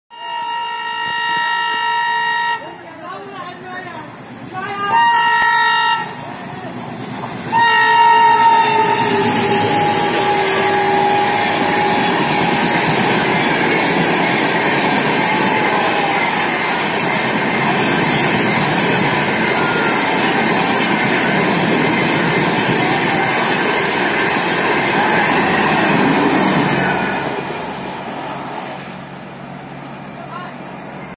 The Alco supremacy | Jaynagar Kolkata furiously skips Ujiarpur
SPJ WDM3A 16655R powering Jaynagar - Kolkata Weekly Express chuggs furiously theough Ujiarpur at 110 kmph